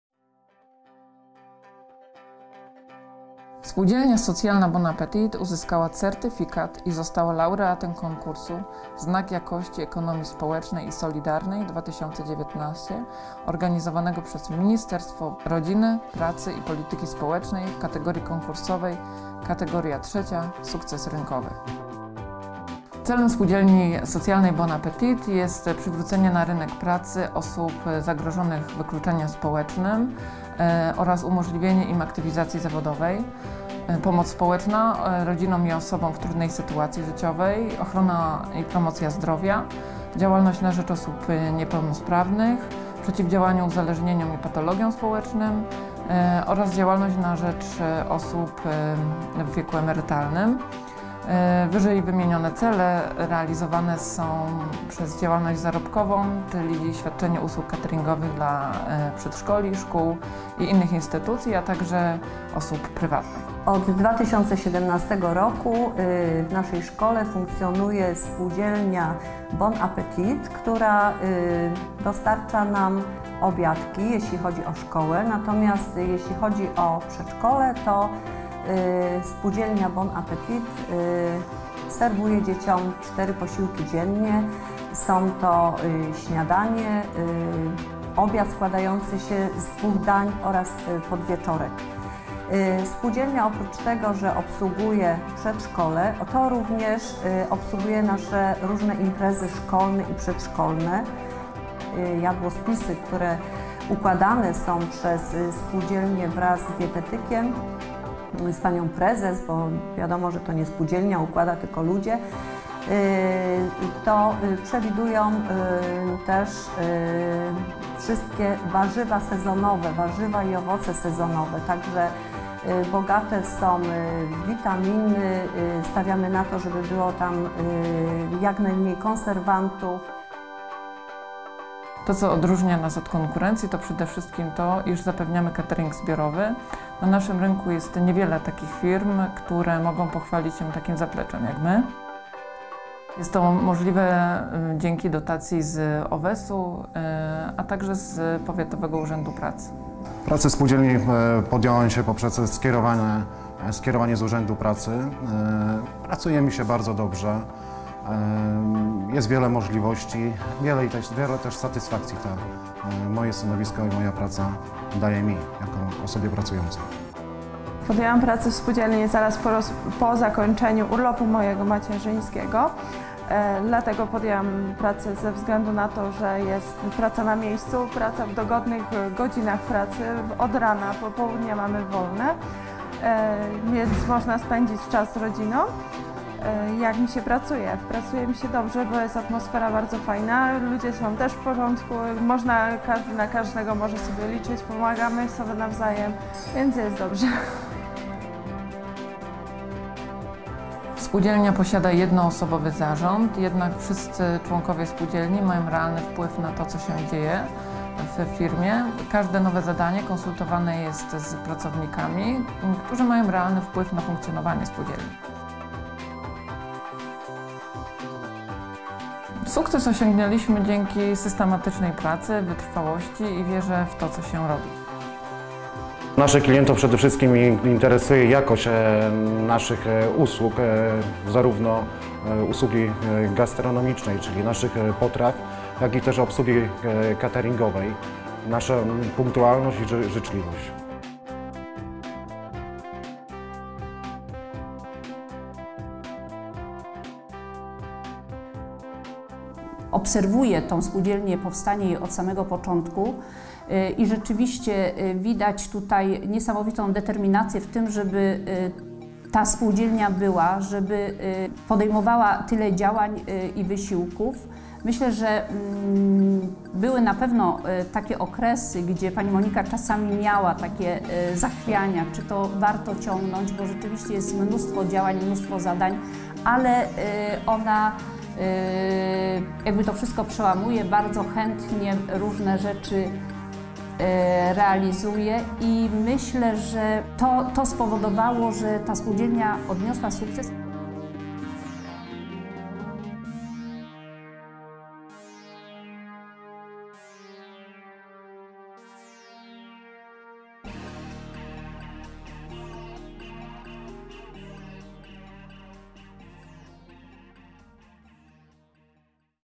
Bon Appetit - to SUKCES rynkowy!
Spółdzielna Socjalna Bon Appetit uzyskała certyfikat i została laureatem konkursu "Znak Jakości Ekonomii Społecznej i Solidarnej 2019" w kategorii "Sukces Rynkowy".

Zapraszamy też na stronę internetową Ministerstwa Rodziny, Pracy i Polityki społecznej